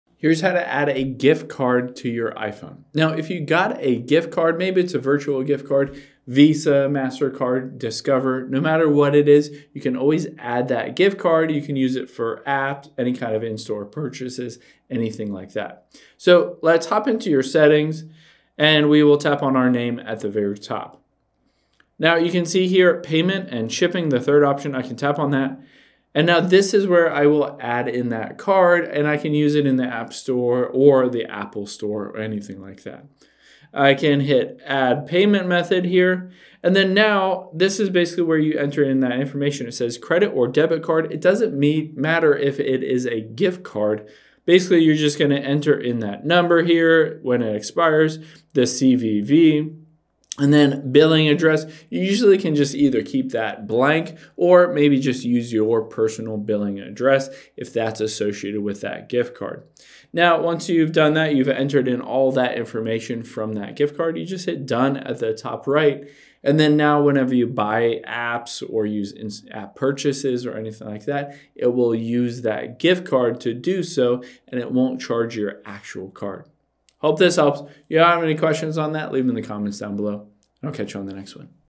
How To Add A Gift Card To iPhone
Let's add a gift card to your iPhone so you can use the card for App Store purchases, in-game purchases, Apple Store, etc.